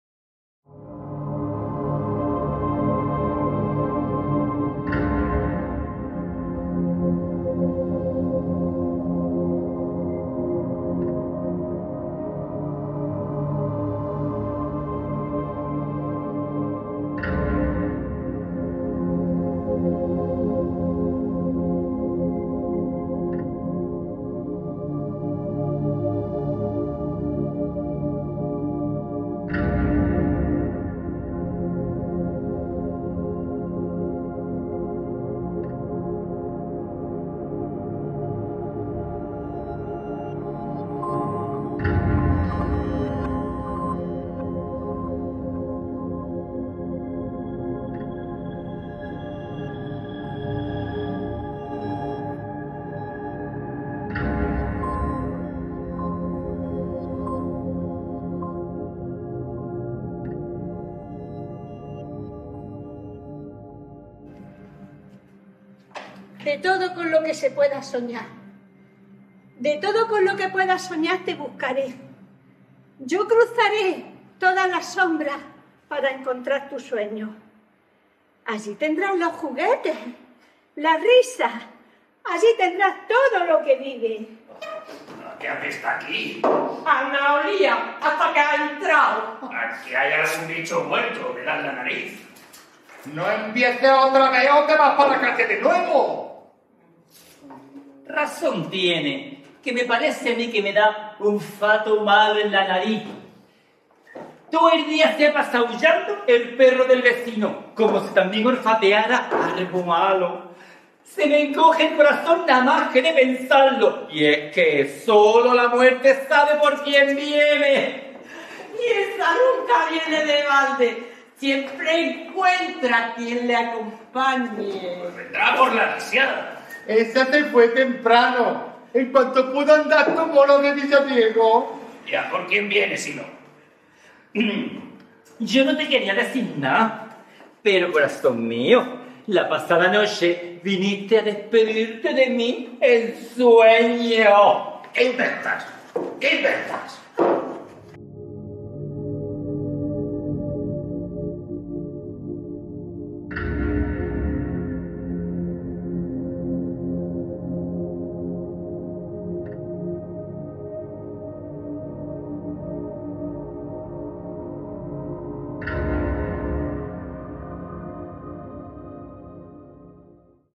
TEASER LITURGIA DE OLVIDOS
Os anticipamos el "teaser" de nuestro próximo estreno: "liturgia de olvidos", un acercamiento personalísimo a "La puerta estrecha", de Eusebio Calonge, desde el respeto y devoción que nos merece el dramaturgo jerezano, que se representará el domingo 3 de septiembre en el Centro Cultural "Alfonso Fernández Torres" de Torreperogil, a las 9 de la noche. Os esperamos.